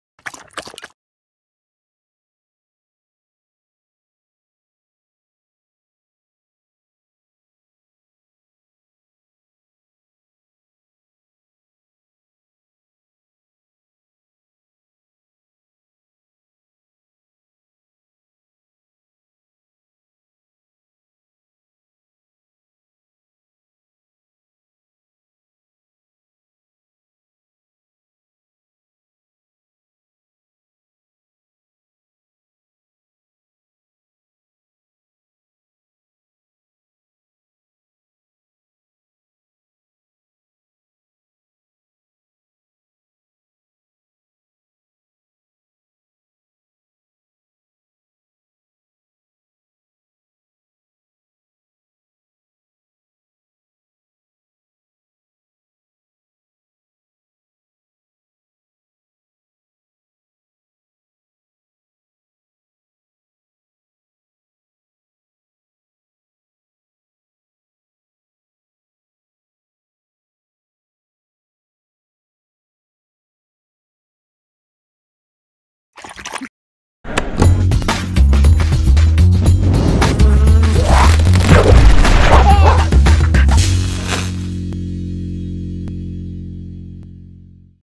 High speed camera illustrates dog drinking in slow motion
Check out this fascinating footage of a dog lapping up some water in super slow motion. So cool!